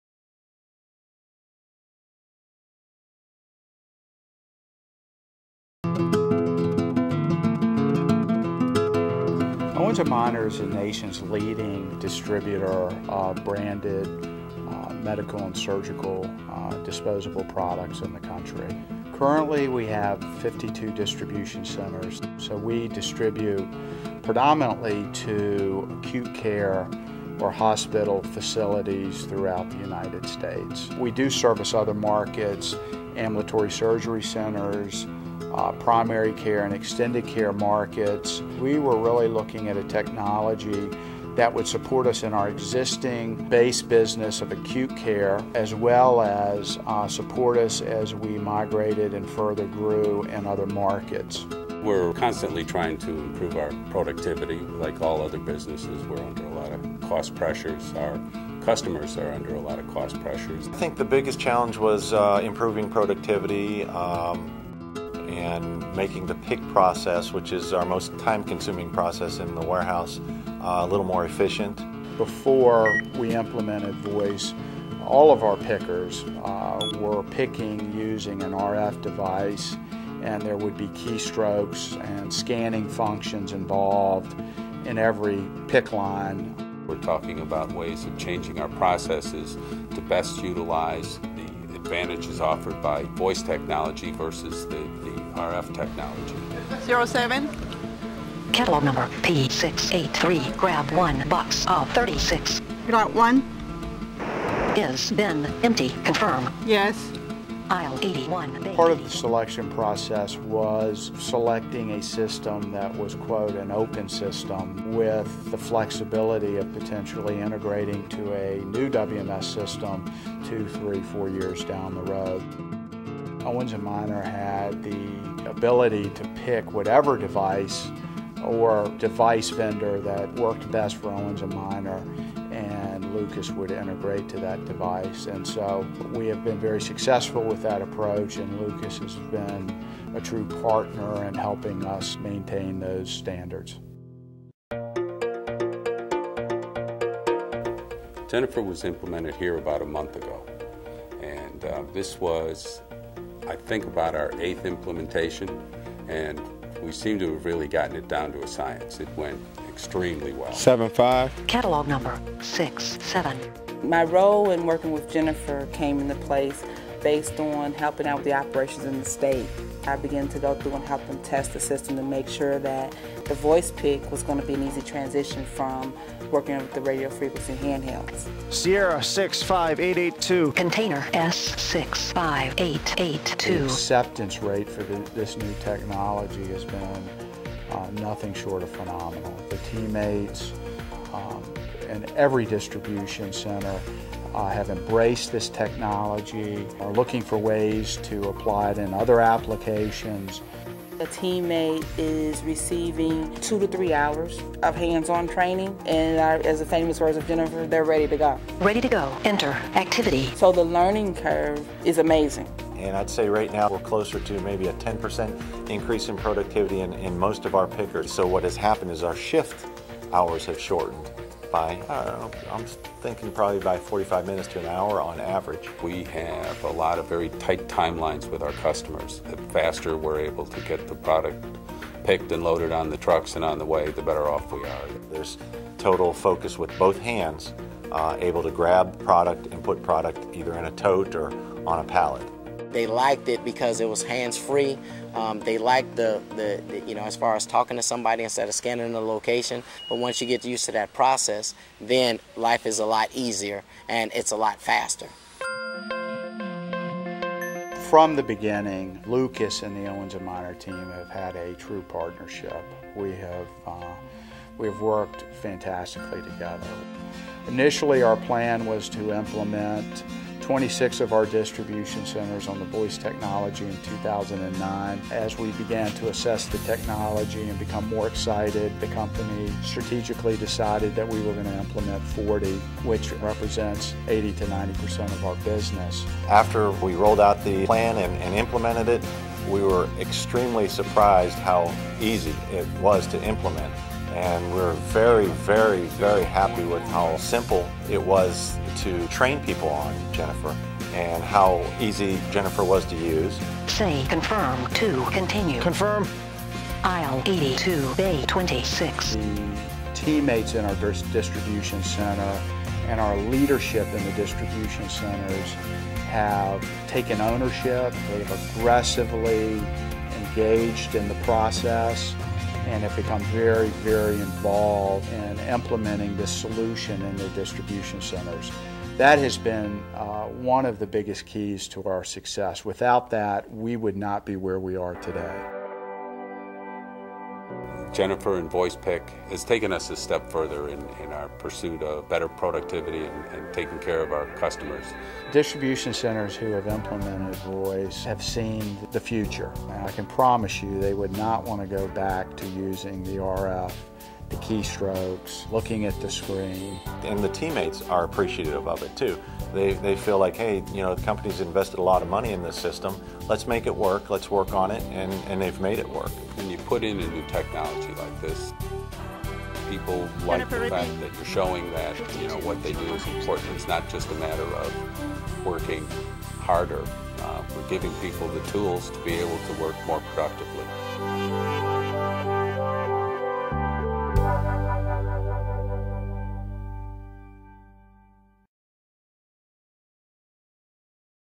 Voice Picking Technology rollout for Owens & Minor's with Jennifer™ AI
Owens & Minor's Rapid Rollout of Jennifer™ Voice Picking Technology

In this video, we explore how medical products distributor Owens & Minor piloted Jennifer™ in February 2009. The initial results were so impressive and user acceptance so fast that Owens & Minor is accelerating the rollout to 40 US distribution centers (DCs) in less than a year, marking the largest and fastest voice picking rollout ever.

🔹 Jennifer™ Pilot Success
Discover the remarkable initial results that led to this rapid expansion, showcasing the efficiency and accuracy gains achieved with Jennifer™.

🔹 Accelerated Rollout

User Acceptance: Learn about the fast user adoption and positive feedback from the pilot.
Expansion Strategy: See how Owens & Minor is implementing Jennifer™ across 40 US DCs, setting a new standard for voice picking technology deployment.
🔹 Key Benefits

Increased Efficiency: Streamline picking processes to boost productivity.
Enhanced Accuracy: Reduce errors and ensure precise order fulfillment.
Scalability: Witness how quickly and effectively the technology can be scaled across multiple locations.
Join us to understand how Owens & Minor is transforming its distribution operations with Jennifer™ and setting a benchmark in the industry for rapid technology adoption.